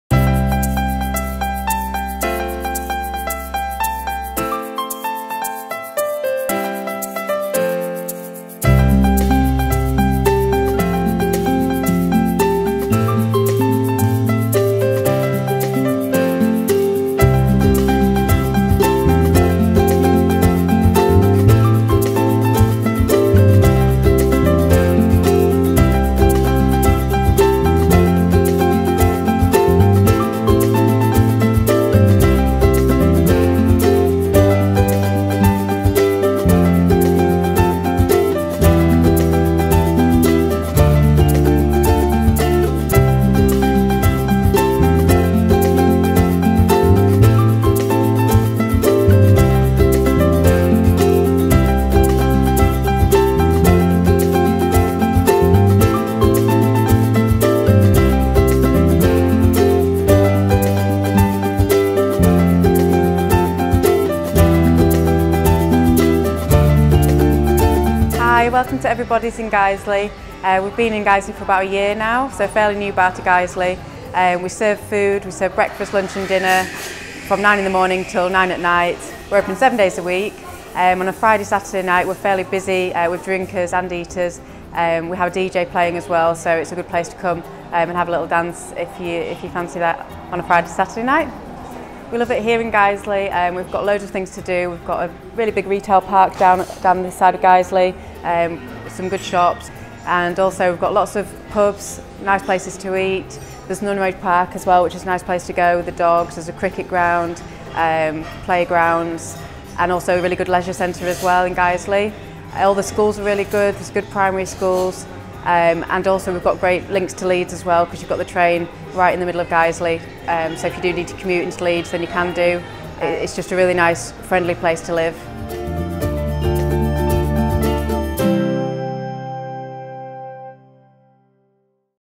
Living in Guiseley, Leeds | Manning Stainton Estate Agents in Guiseley
Our guide to living in Guiseley, as the local property expert we want to showcase Guiseley and the reasons to choose Guiseley as your next place to live in Leeds. More detail can be found at

Start your Search